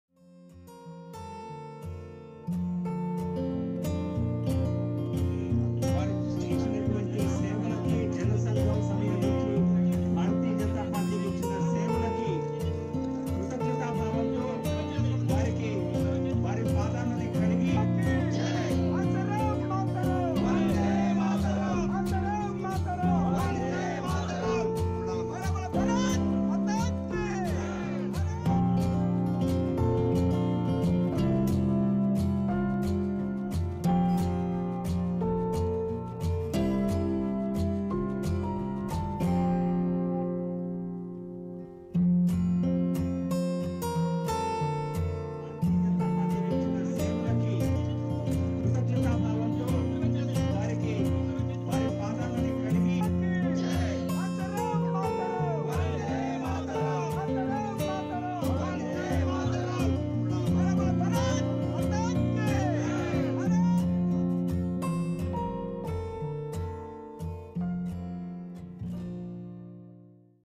Watch: Shivraj Singh washes feet of Jan Sangh workers in Vijayawada
Vijayawada (AP), July 14 (ANI): Former chief minister of Madhya Pradesh, Shivraj Singh Chouhan washed the feet of Jan Sangh workers in Andhra Pradesh’s Vijayawada.